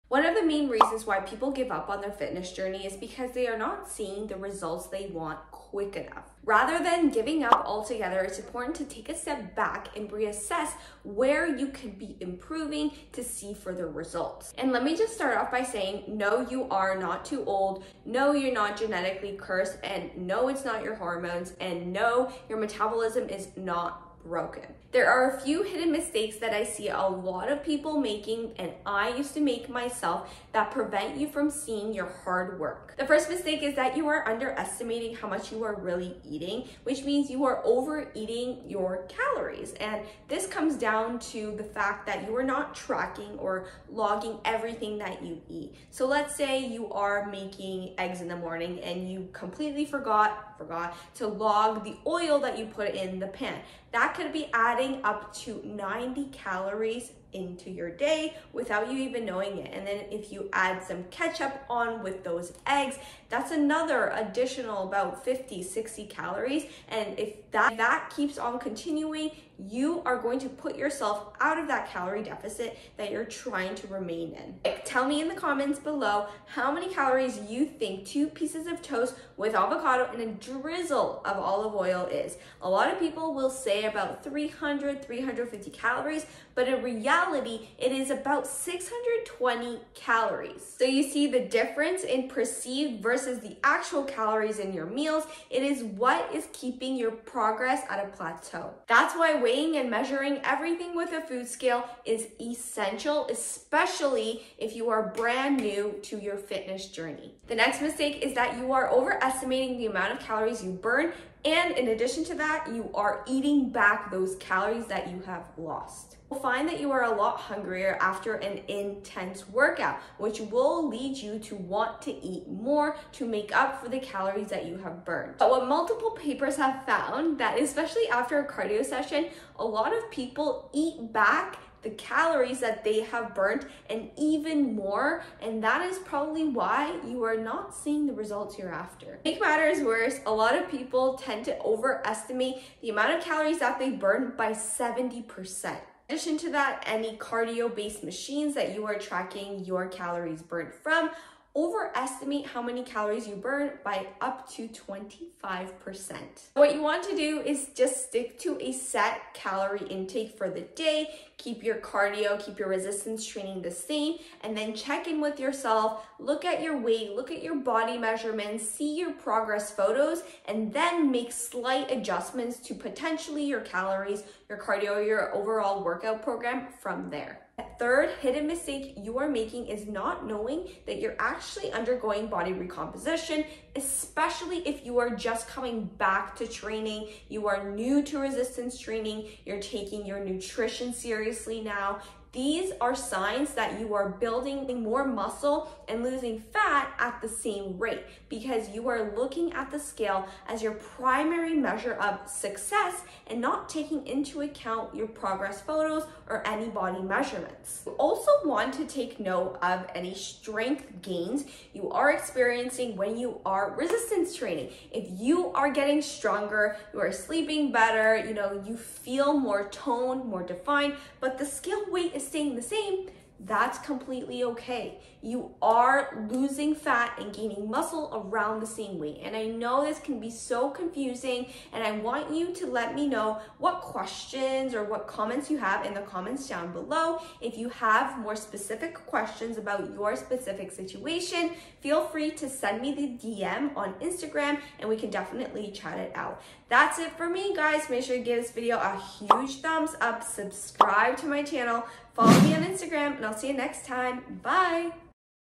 3 Common Reasons You're Not Losing Weight
You invest lots of time and effort into understanding how to lose fat, yet the outcomes often remain elusive, leading to discouragement and quitting your fitness journey.
Correcting some common mistakes that I explain in this videsee will have your progress skyrocketing!